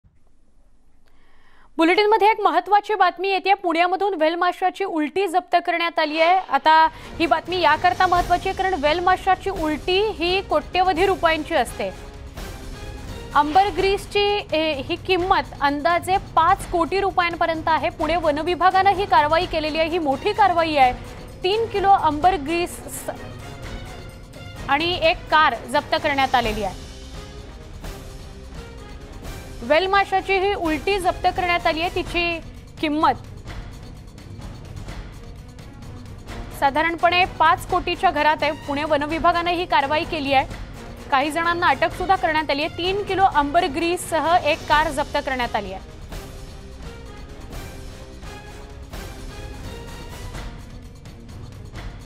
Pune Crime | पुणे वनविभागाची मोठी कारवाई, पुण्यात व्हेल माशाची उलटी जप्त तर 6 आरोपींना अटक
पुण्यातून  व्हेल माशाची उलटी जप्त करण्यात आलेली आहे. व्हेल माशाची उलटी ही कोठ्यवधी रुपयांची असते. अंबरग्रीस  किमत अंदाजे 5 कोटी रुपयांपर्यत आहे. पुणे वनविभागाची ही मोठी कारवाई आहे.